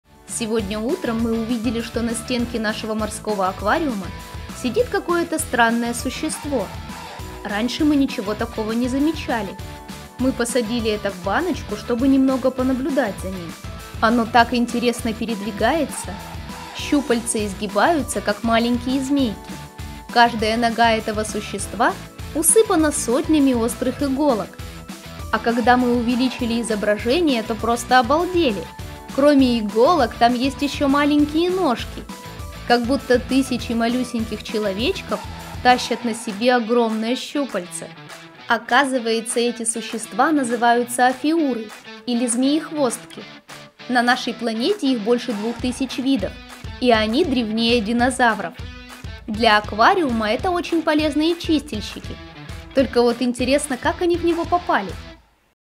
Как это существо попало в аквариум? Офиуры. Животные пережившие динозавров.
В нашем морском аквариуме мы заметили странное существо.  Оказалось, что это офиуры - животные, пережившие динозавтров. Но как оно попало в наш аквариум?!